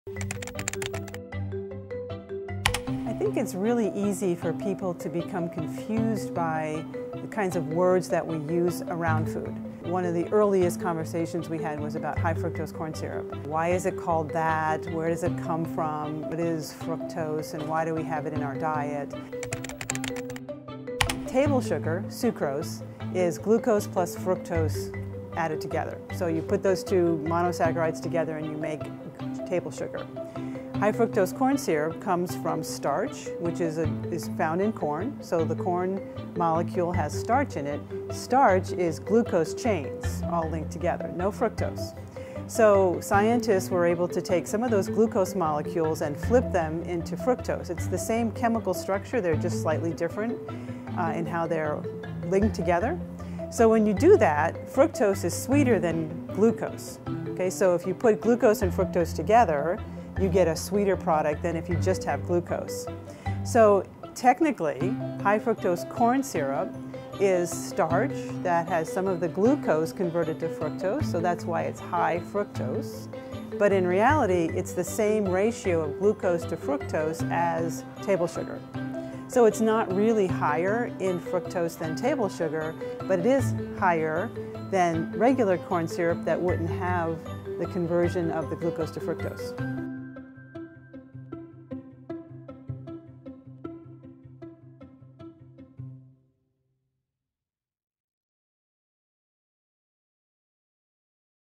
Food Myths 101: Fructose
Dr. Ruth MacDonald explains the makeup of fructose and high fructose corn syrup and debunks misconceptions.